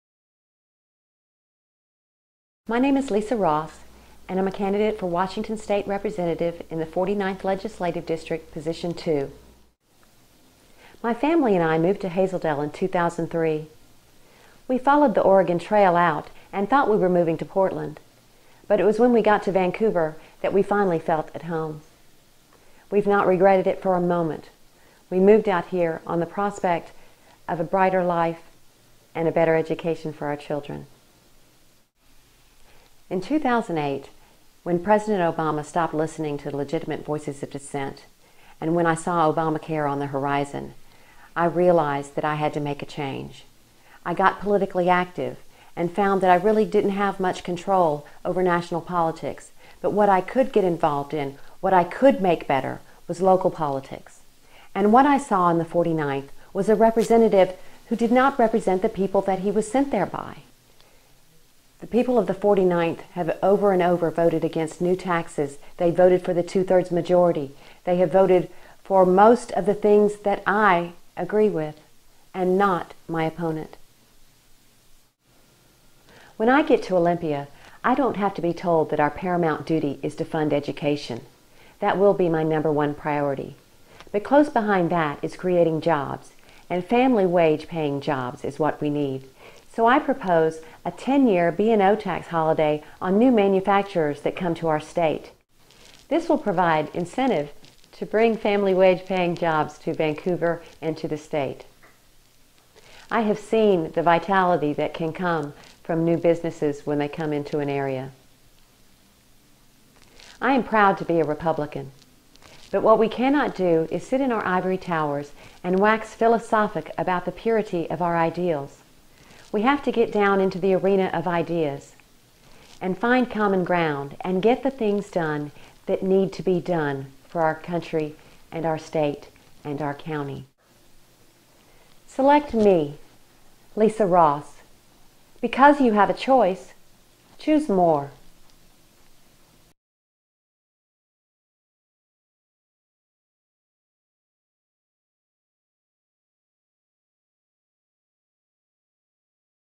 Introducing Lisa Ross 1
Lisa is a candidate for Washington State Representative Position 2 in the 49th Legislative District